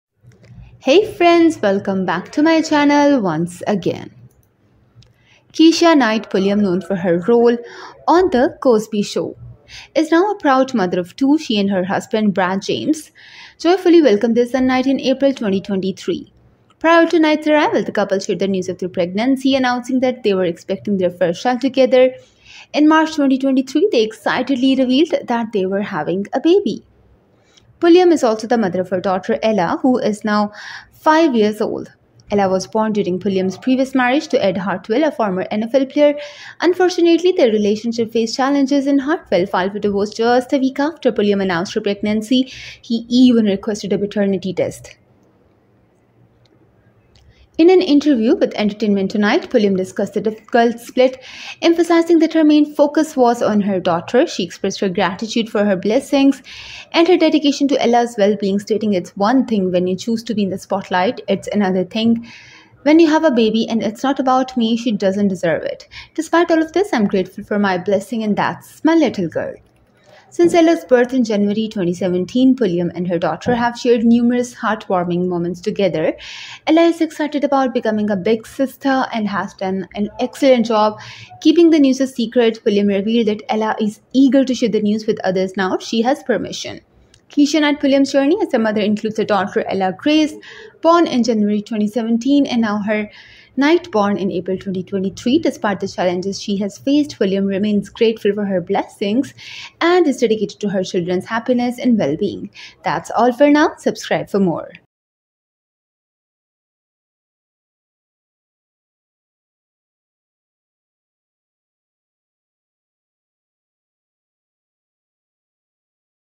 Keshia Knight Pulliam Heart-Touching Moment In Video Of Her Son When He Spending Quality Time!🥰
Keshia Knight Pulliam, known for her role on The Cosby Show, is now a proud mother of two. She and her husband, Brad James, joyfully welcomed their son, Knight, in April 2023.
Prior to Knight's arrival, the couple shared the news of their pregnancy, announcing that they were expecting their first child together. In March 2023, they excitedly revealed that they were having a baby boy.